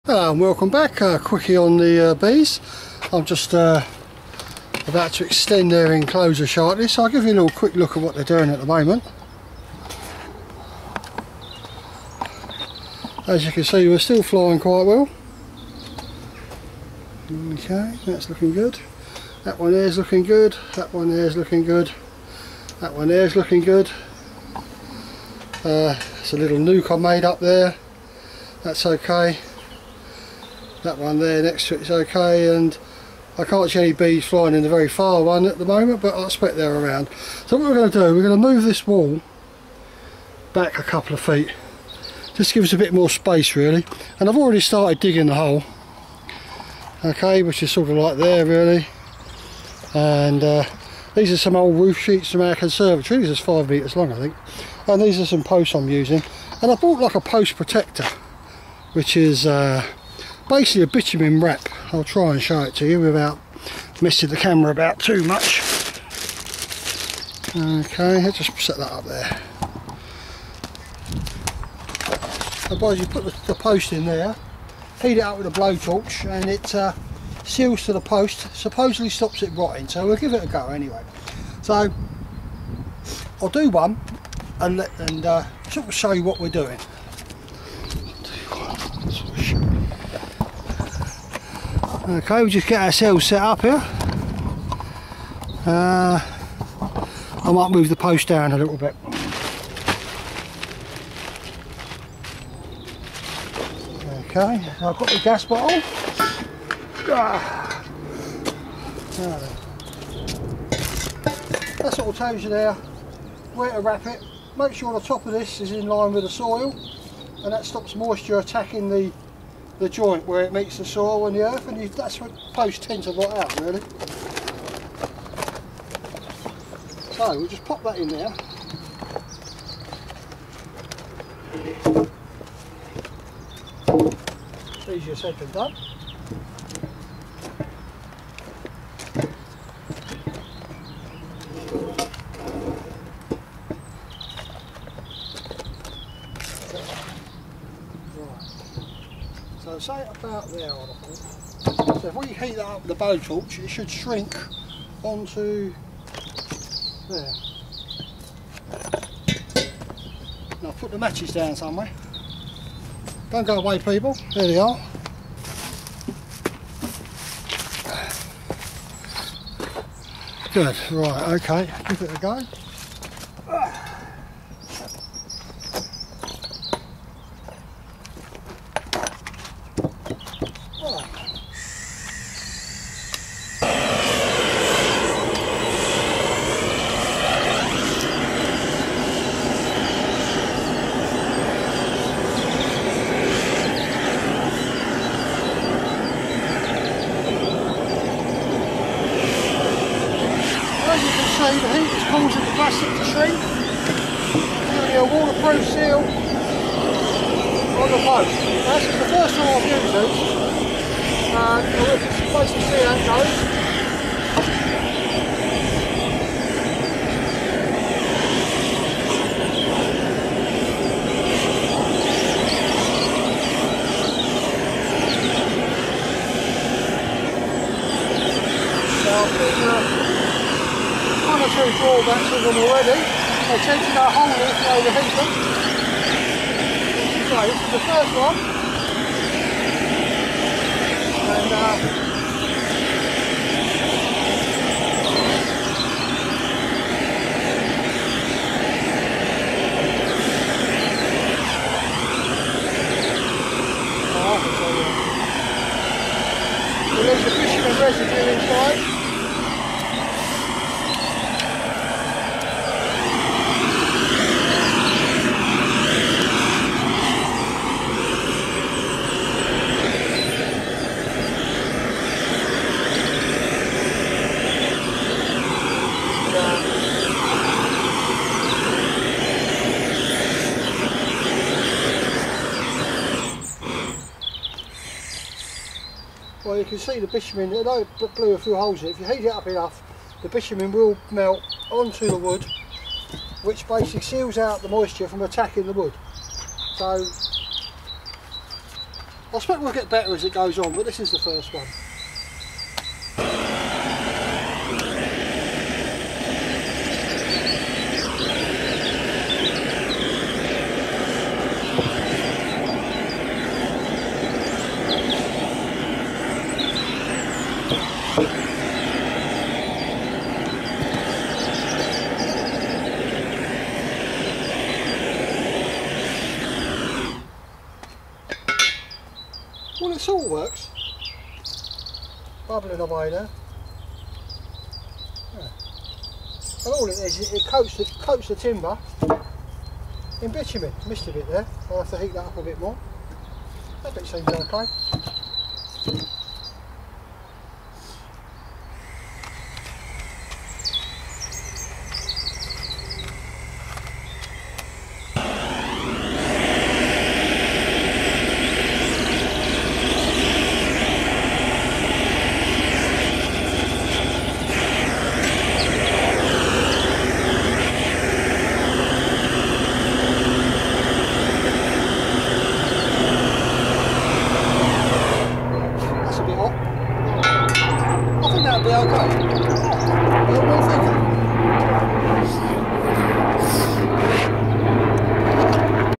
beekeeping alterations to there space plus putting up fence posts part 1
Basic guide on planting veg plants flowers also beekeeping and other interesting things military vehicles diy ect